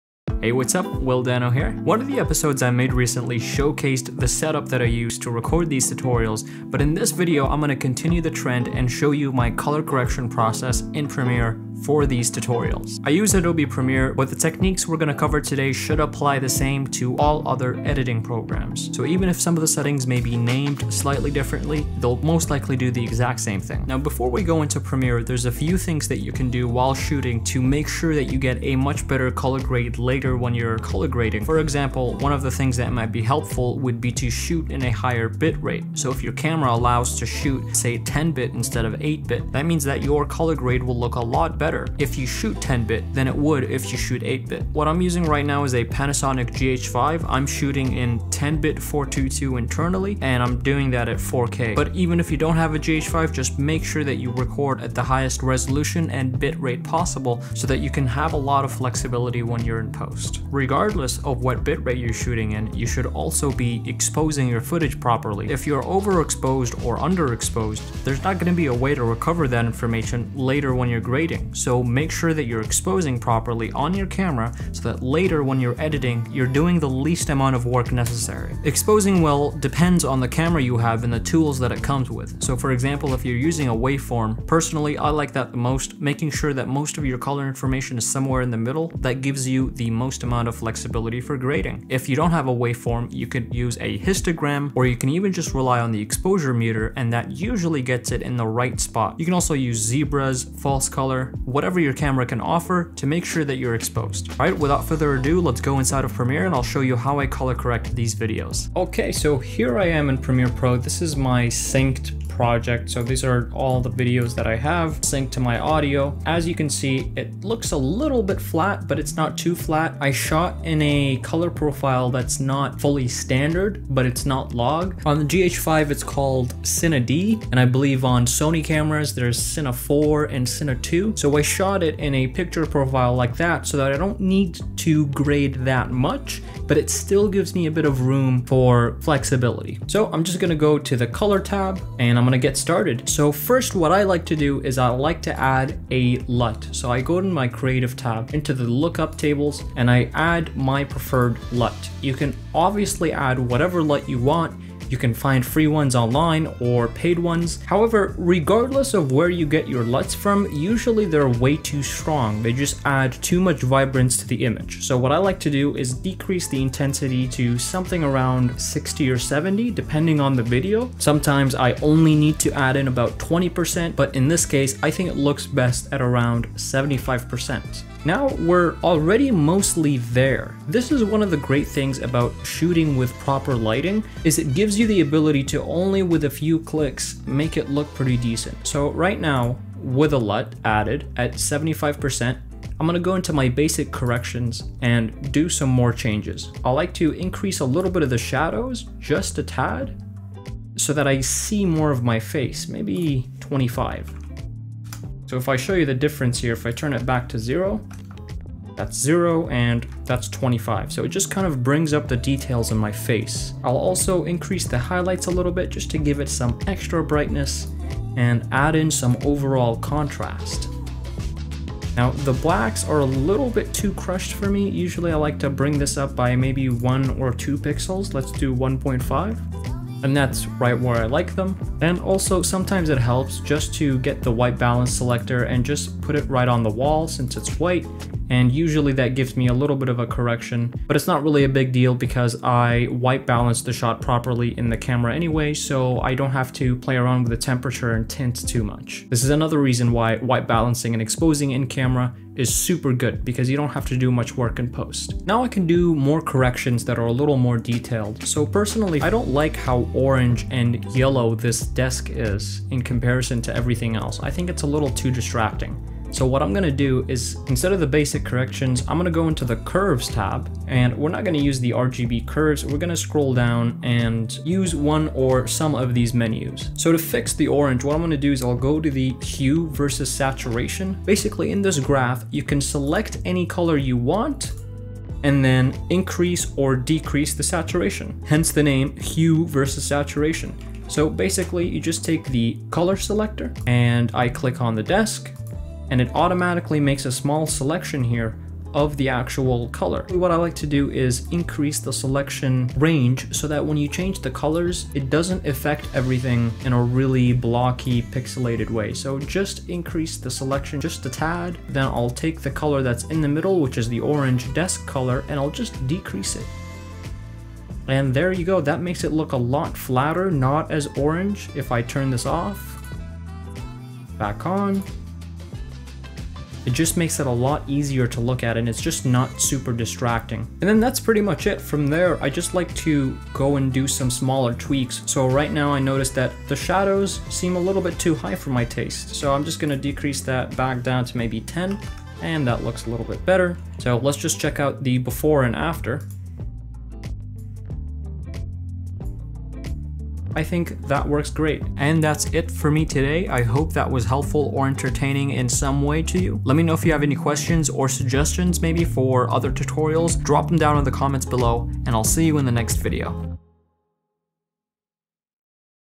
A Beginners Guide to Color Grading
Today Will shows how anyone can color grade their videos with just a few easy steps. This tutorial is great for anyone new to grading that wants to give their video that extra touch of color.